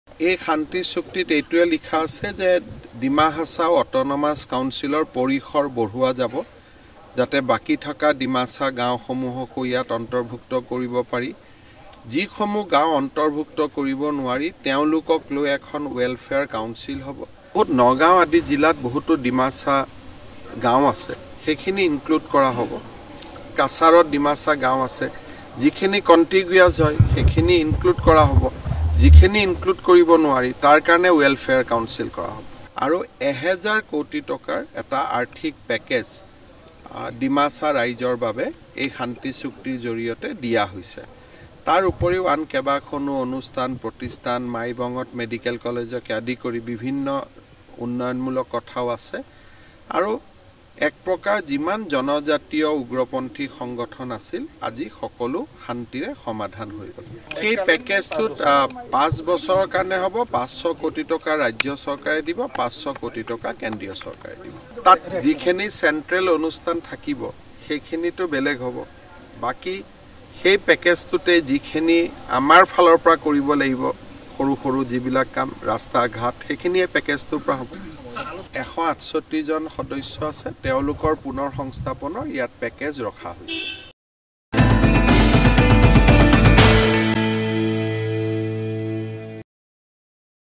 ডিমাচা জাতিৰ উন্নয়নৰ বাবে ১ হাজাৰ কোটি টকাৰ পেকেজ চৰকাৰৰ
🔴 বৃদ্ধি কৰা হ'ব ডিমাচা স্বায়ত্ব পৰিষদৰ পৰিসৰ
🔴 বিভিন্ন জিলাত থকা ডিমাচা সকলৰ বাবে গঠন কৰা হ'ব উন্নয়ন পৰিষদ
🔴 চুক্তি স্বাক্ষৰৰ পিছত জনালে মুখ্যমন্ত্ৰী হিমন্ত বিশ্ব শৰ্মাই